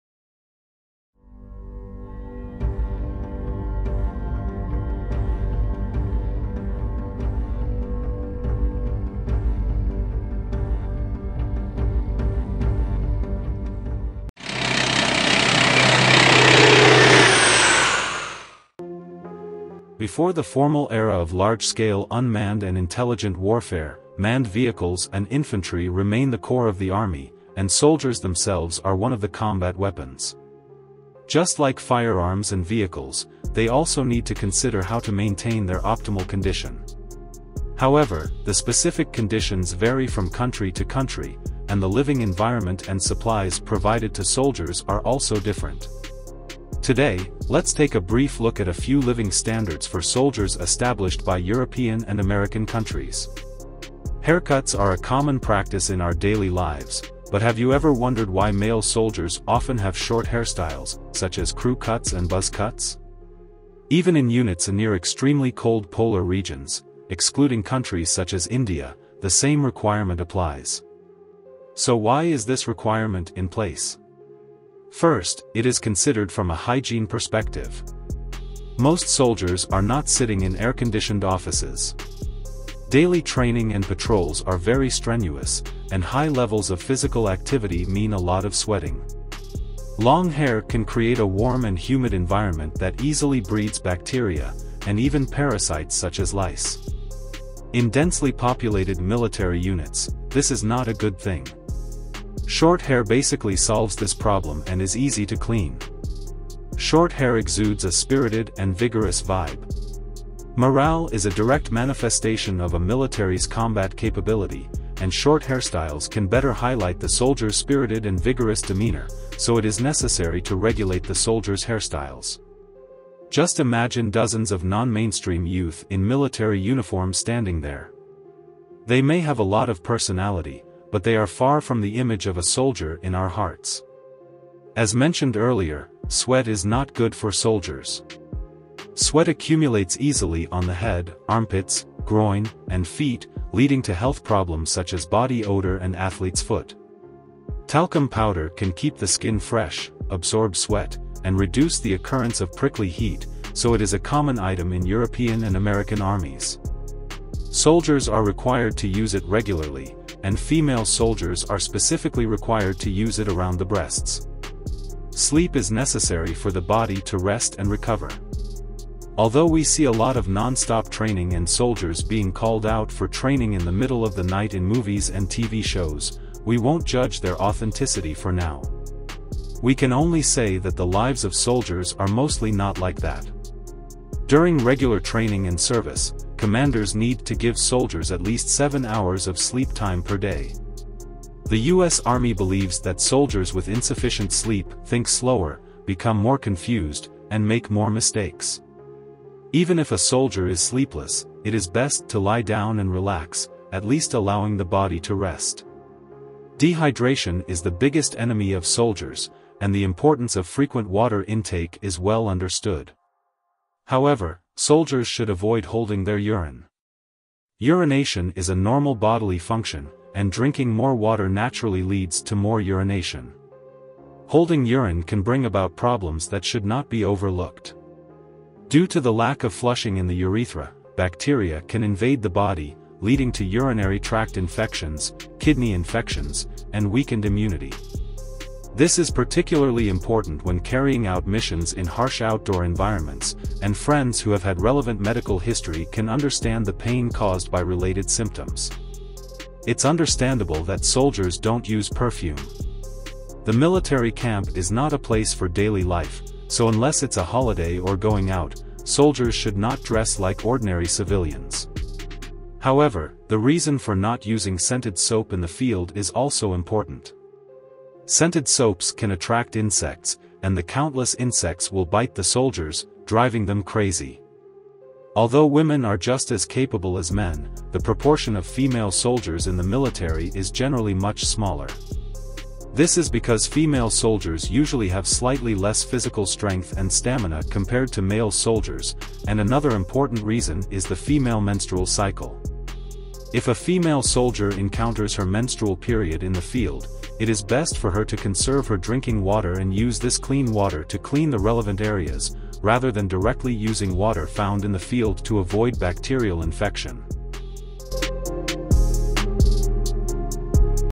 Enhancing Soldier Performance: Essential Gear and Hygiene Standards
The video discusses the living standards for soldiers in European and American countries, including the requirement for short hair for hygiene and morale, the use of talcum powder to absorb sweat, the importance of sleep, the need for frequent water intake, and the challenges female soldiers face during their menstrual cycle in the field.

The living standards for soldiers in European and American countries include requirements for short hair for hygiene and morale, the use of talcum powder to absorb sweat, the importance of sleep, the need for frequent water intake, and the challenges female soldiers face during their menstrual cycle in the field.